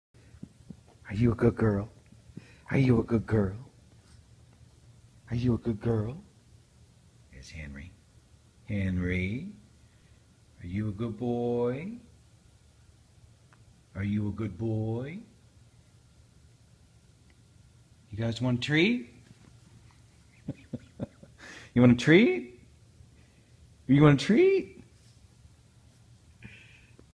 The Tilting Heads
Henry and Rat Dog become bobbleheads.